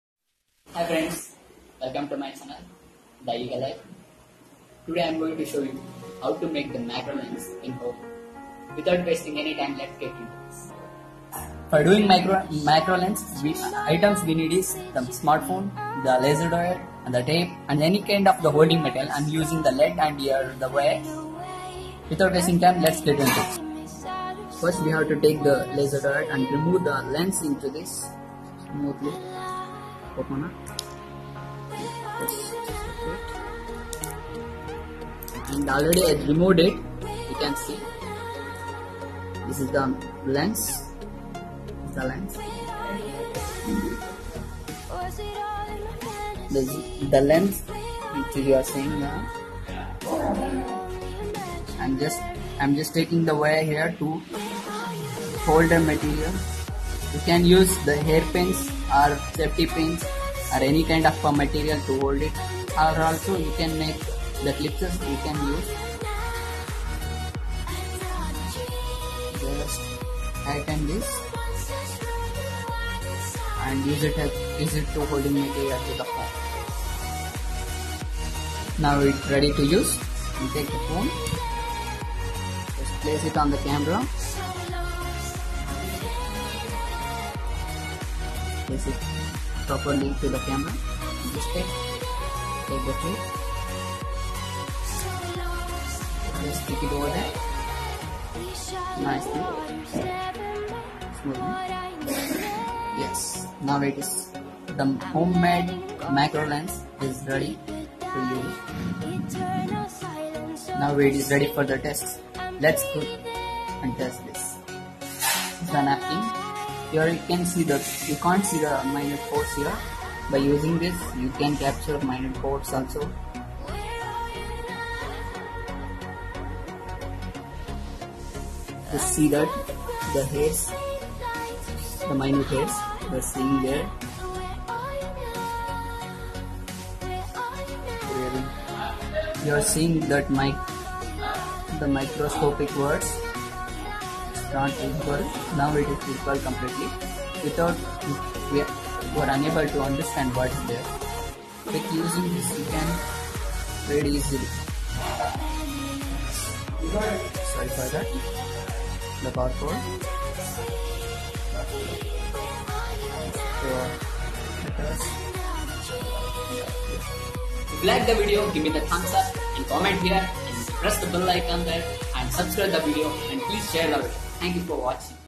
How to make macro lens for mobile (home made)
awesome macro lens for mobile with low price. Very easy to make with in minutes, very handy, very easy to use. The great mobile accessory. This  mobile accessory highly recommanded for focasing and zooming micro particles.